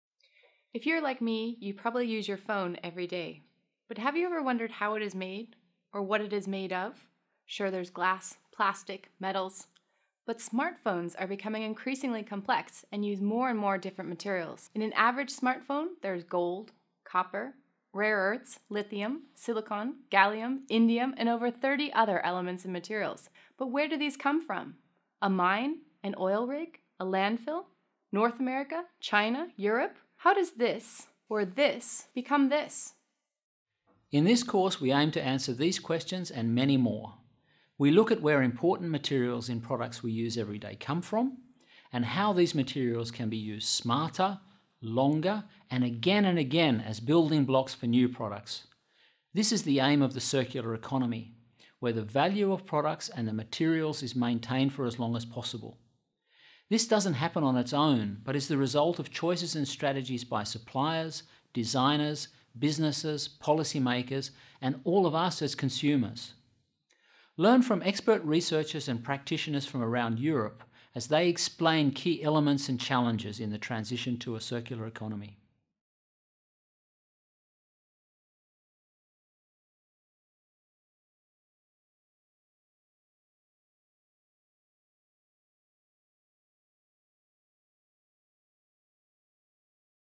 New MOOC on Circular Economy
‘Circular Economy: sustainable materials management’ is a FREE online course available and open to everyone on Coursera from November 2018.